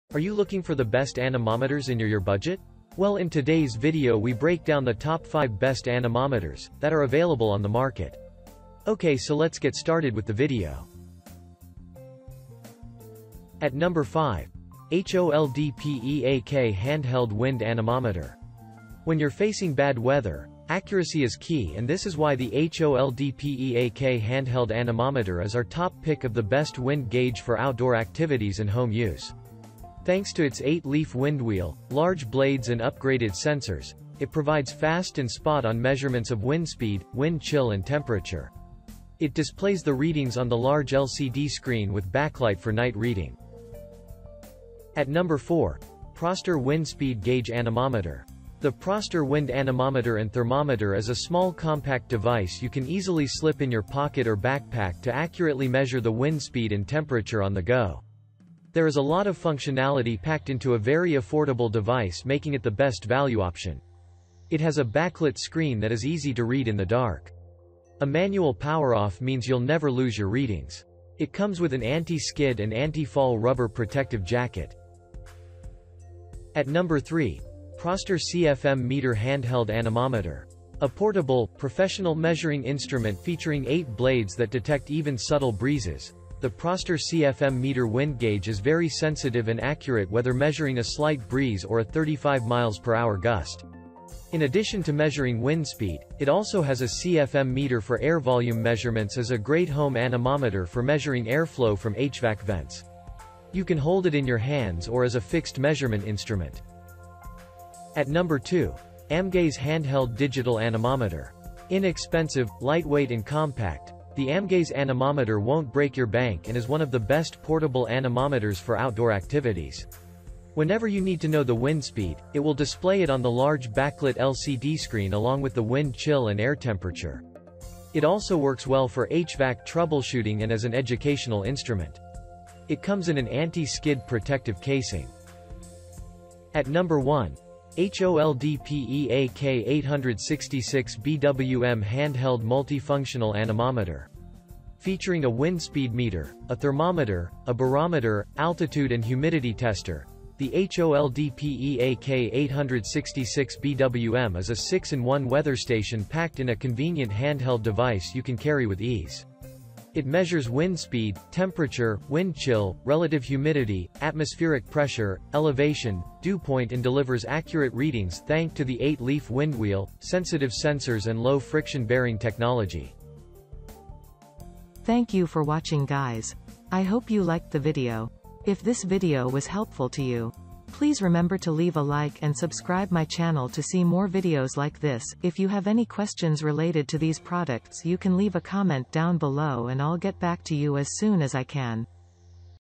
The 5 Best Anemometers In 2021
☛ All The Links to Best Anemometers listed in this Video:-

►5. HOLDPEAK Handheld Wind Anemometer.

► 4. Proster Wind Speed Gauge Anemometer.

► 3.Proster CFM Meter Handheld Anemometer.

► 2.Amgaze Handheld Digital Anemometer.

►1.HOLDPEAK 866B-WM Handheld Multifunctional Anemometer.